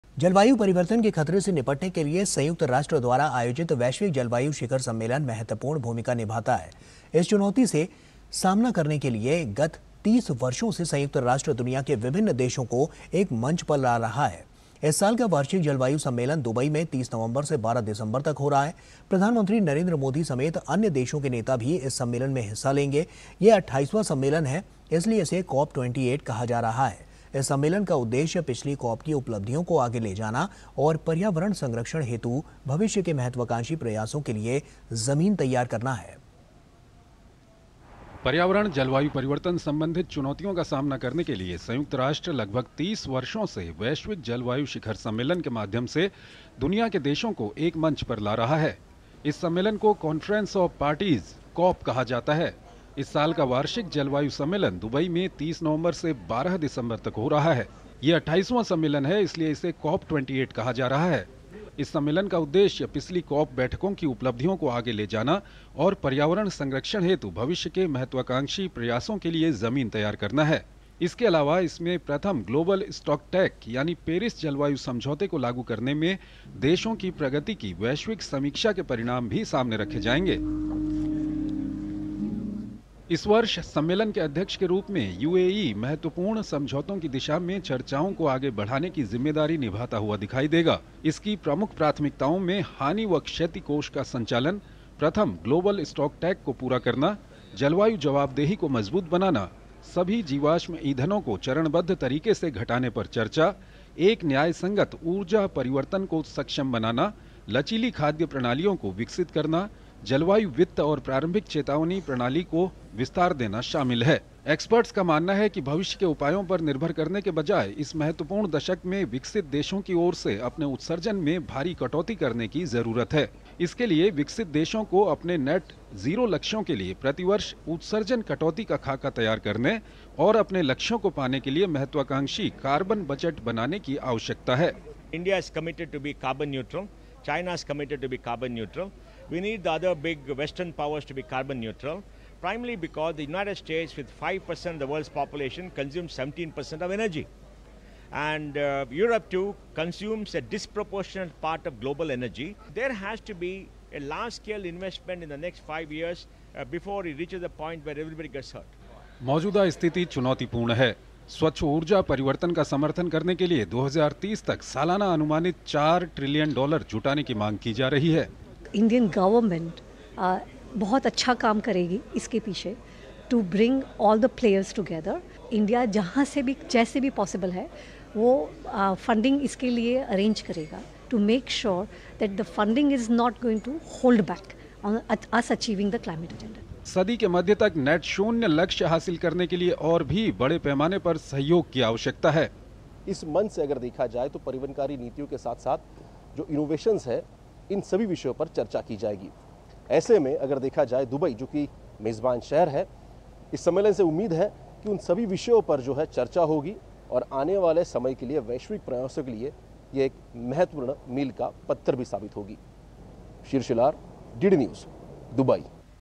30 नवंबर से 12 दिसंबर तक दुबई में आयोजित होगा वार्षिक जलवायु सम्मलेन COP 28
30 नवंबर से 12 दिसंबर तक दुबई में आयोजित होगा वार्षिक जलवायु सम्मलेन COP 28